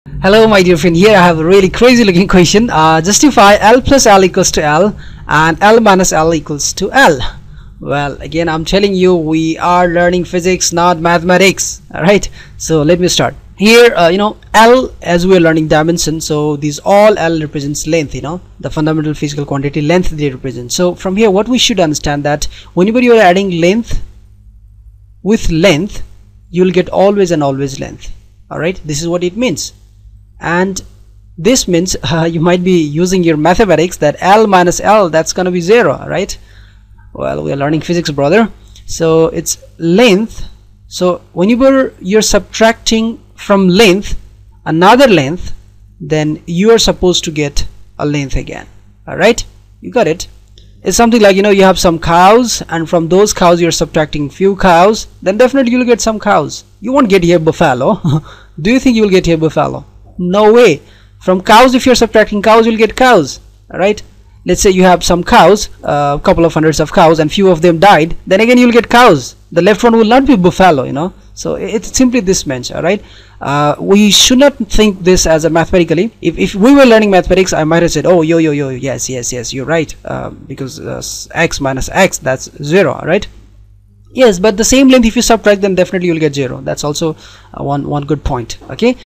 Justification Question in Dimensional Analysis
Justifying Answer video for Dimensional Analysis question. This Dimension question simply means when we add length with length we get length and when we subtract length from length we get length. That's the simple concept of physics hidden in this question. We are discussing sitting on the frame of Physics rather than mathematics , so from physics, point of view its true may be wrong to justify from mathematical point of view.